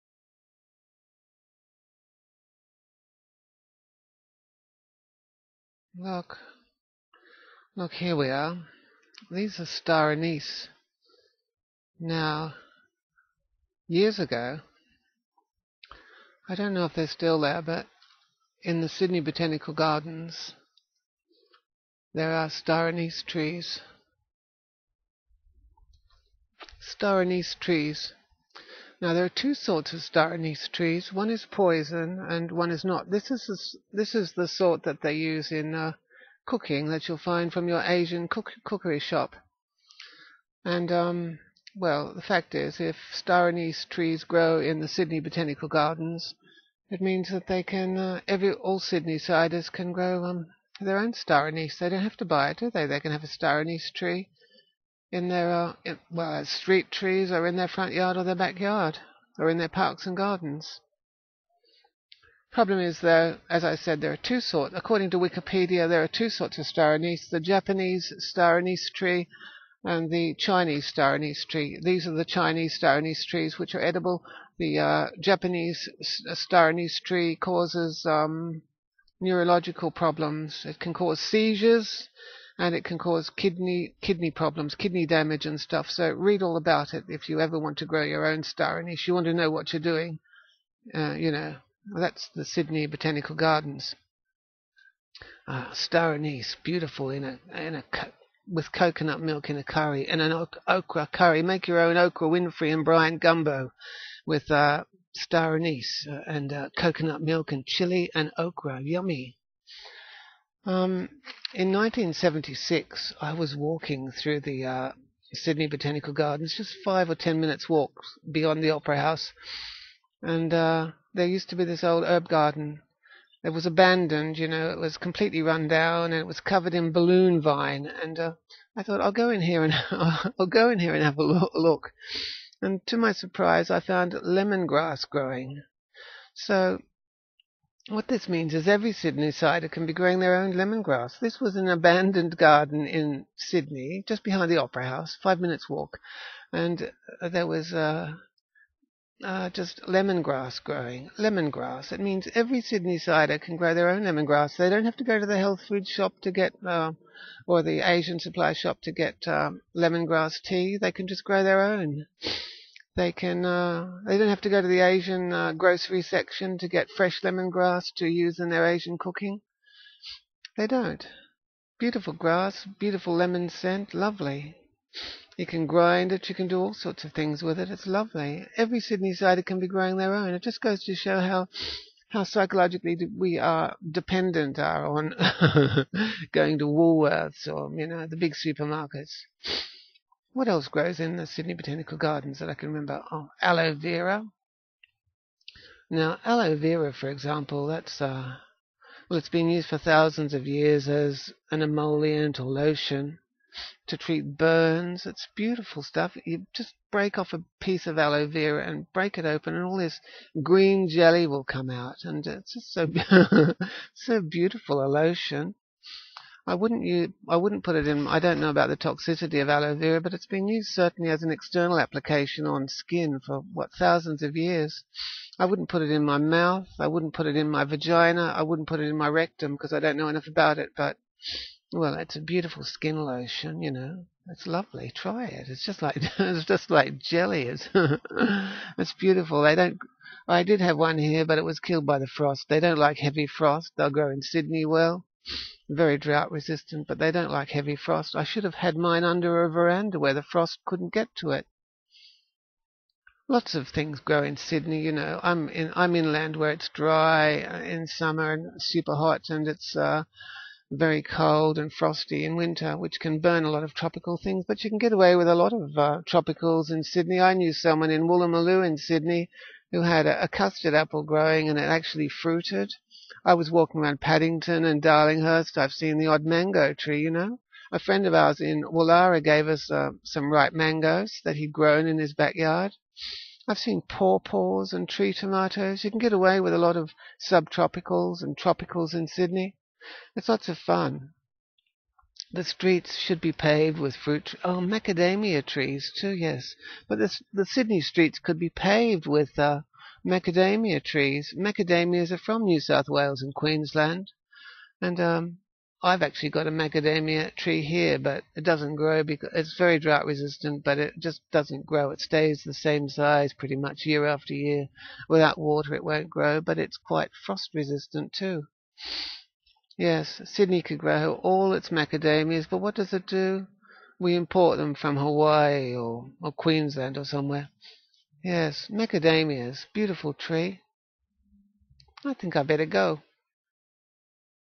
Sydney Opera House Permaculture
Discussing useful plants found in the Sydney Botanical Gardens--such as star anise, lemon grass and aloe vera.

There are TWO sorts of star anise trees, Chinese and Japanese. The Japanese is the toxic one that can cause seizures and other problems. The Chinese is the edible one. Which sort grows in the Sydney Botanical Gardens?

Lemongrass is useful for making beverages and, also, is used in Asian cooking.

Aloe vera yields a green jelly which has been used as an emollient for thousands of years. But can it be used as a personal lubricant? Or is it toxic when used internally?

P.S.

Since making this video I have learned that people use Aloe vera gel as food in "smoothies" made in the blender. There are quite a few YouTube videos about how to fillet the Aloe vera leaves and extract and wash the inner gel, which is less bitter. Also, here is a great video on a Japanese type of Aloe that is not bitter, at all, and can, apparently, be eaten skin and all. Chomp! Chomp! :